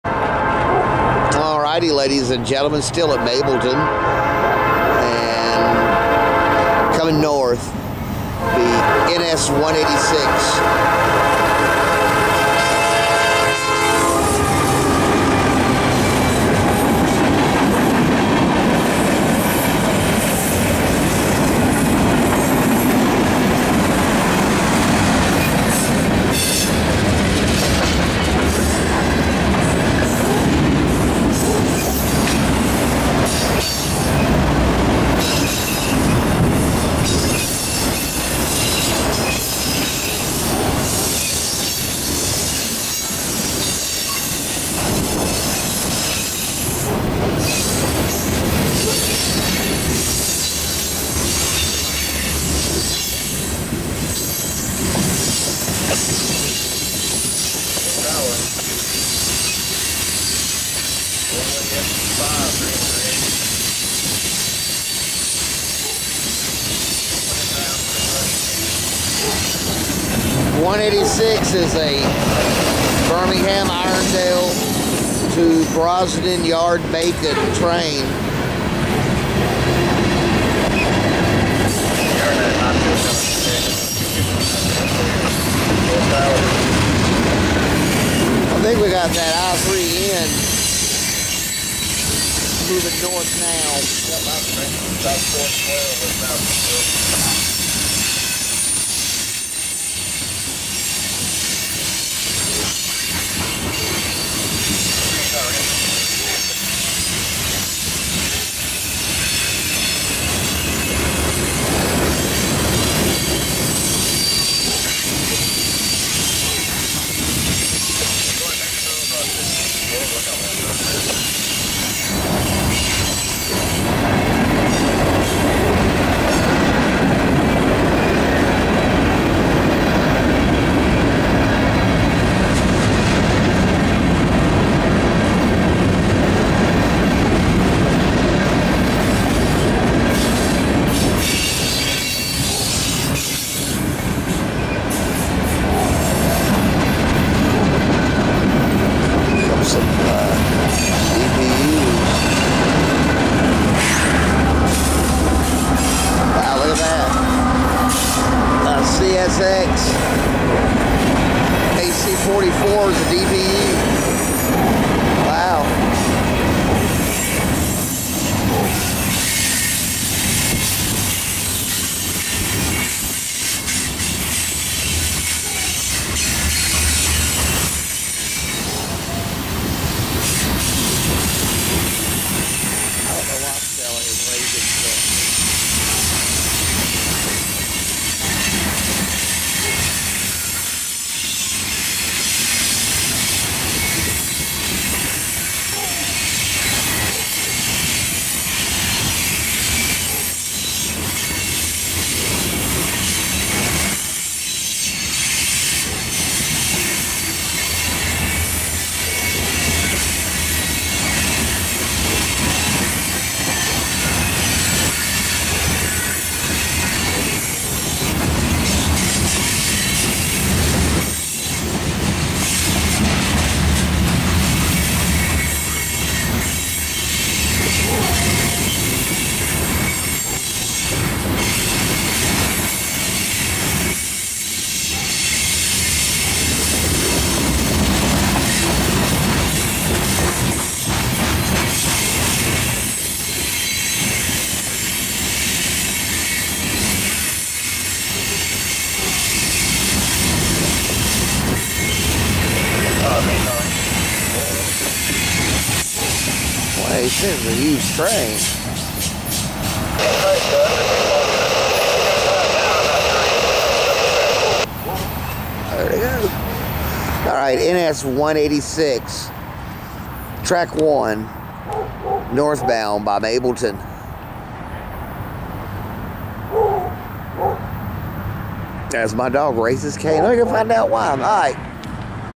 NS 186 Manifest by Mableton 4-1-21 long train CSX AC44 in DPU set
Irondale to Macon manifest train NS 186 powers north by Mableton. 186 usually had a long train before PSR, and that hasn't changed.  A CSX AC44 will be included in the Mid train DPU set.

NS Coastal Div
Atlanta Terminal
MP 137.0 H
Mableton Ga
AT Disp Ch 42